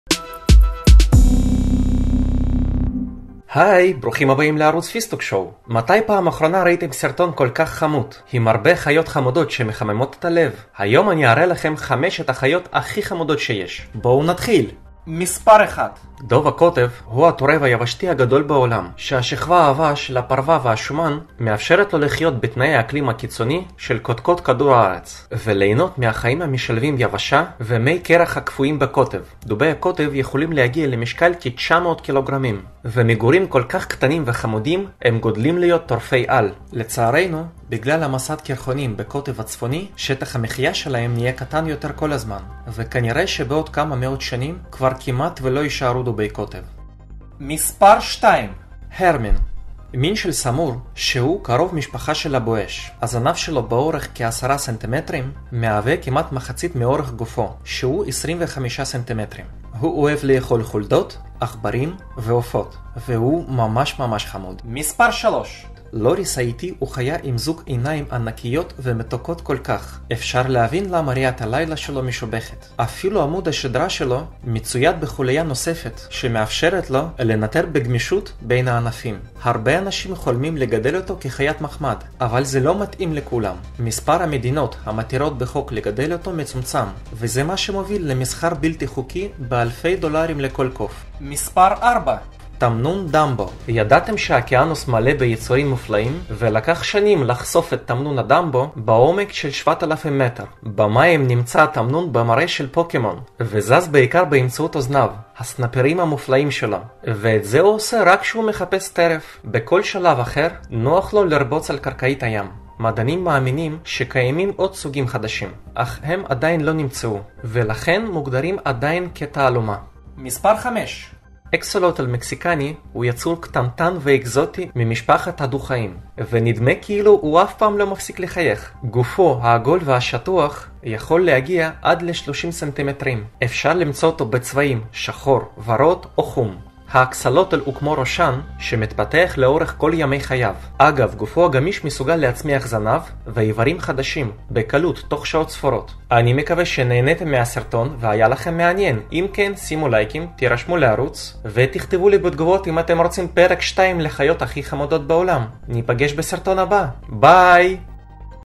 טופ 5 חיות הכי חמודות בעולם
היום אציג לכם טופ 5 חיות הכי חמודות בעולם.